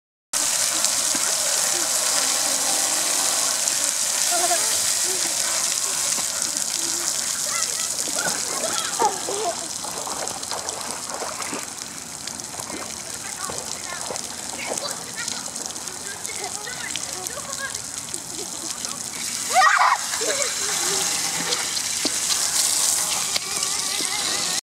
Looking down the pipe not expecting a Water blast in face
peeping into water outlet in the park!